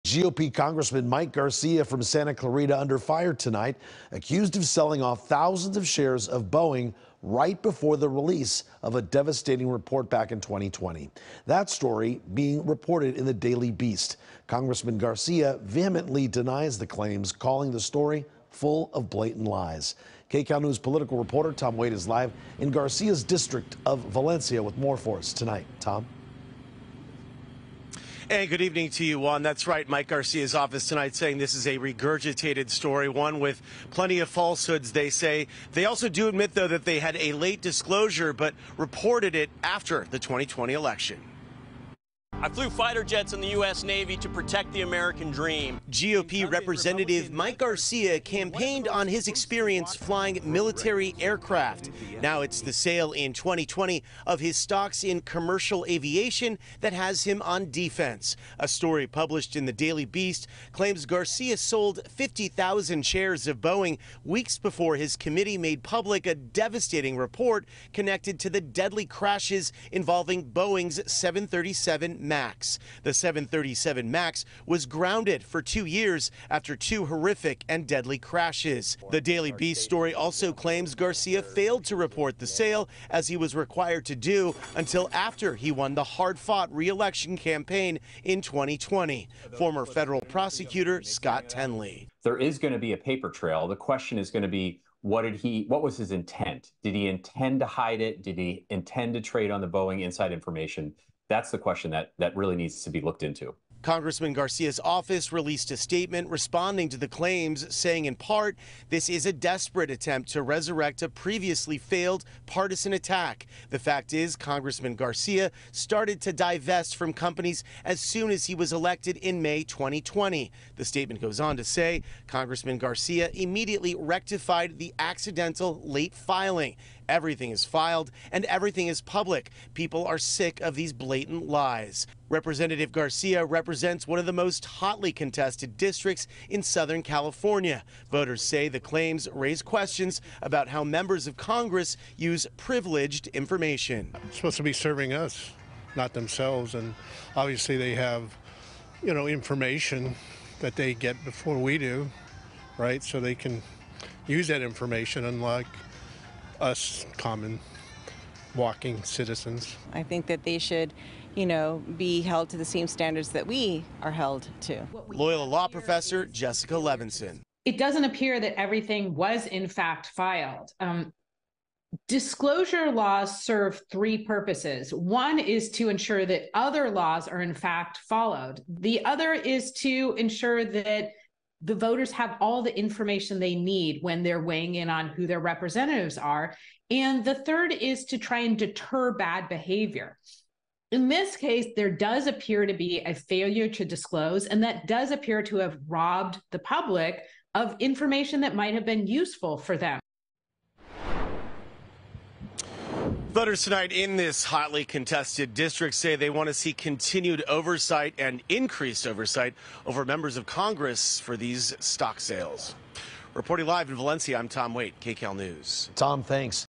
Congressman Garcia accused of selling Boeing stock before devastating report
Republican Congressman Mike Garcia is accused of selling off thousands of shares of Boeing right before he release of a devastating report in 2020. Tom Wait reports.